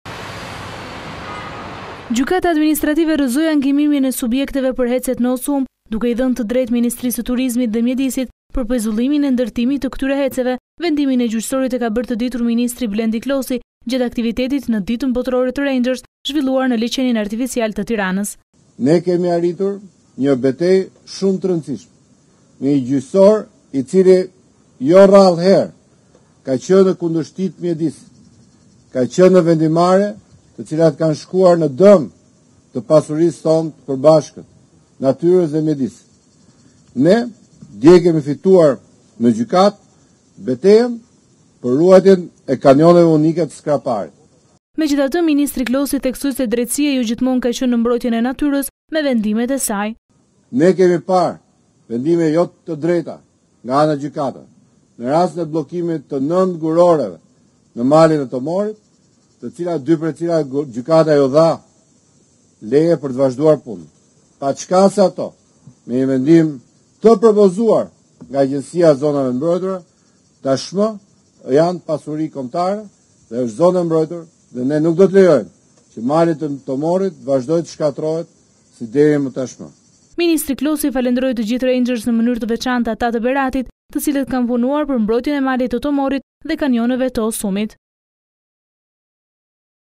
Gjykata rrëzon HEC-et në kanionet e Osumit, Blendi Klosi: Shpëtoi perla e natyrës
Gjykata Administrative rrëzoi ankimimin e subjekteve për HEC-et në Osum duke i dhënë të drejtë Ministrisë së Turizmit dhe Mjedisit për pezullimin e ndërtimit të këtyre HEC-eve, disa muaj më parë. Vendimin e gjyqësorit e ka bërë të ditur ministri Blendi Klosi gjatë aktivitetit në Ditën Botërore të Rangers, zhvilluar në Liqenin Artificial në Tiranë.

Fituam një nga betejat e para për natyrën. Gjykata sapo ka vendosur, anullimin e HEC-eve Osum.

Deklaroi ministri Klosi gjatë fjalës së tij
Kreu i Turizmit dhe Mjedisit përshëndeti vendimin e Gjykatës duke u shprehur se ky vendim: “Na nxit edhe më shumë për të vijuar betejën tonë nē mbrojtje të natyrës. Më në fund, perla natyrore e Osumit, shpëtoi. Osumi do të mbetet pasuri e përbashkët natyrore”.

Megjithatë ministri Klosi theksoi se drejtësia jo gjithmonë ka qenë në mbrojtjen e natyrës me vendimet e saj.

Pak ditë më parë, gjykata dha vendimin skandaloz tërifillimit nga puna të dy guroreve në Berat. Por pak nga pak dhe të gjithë bashkë do t’ia dalim të mbrojmë asetet tona natyrore.

Blendi Klosi, ministër i Mjedisit dhe Turizmit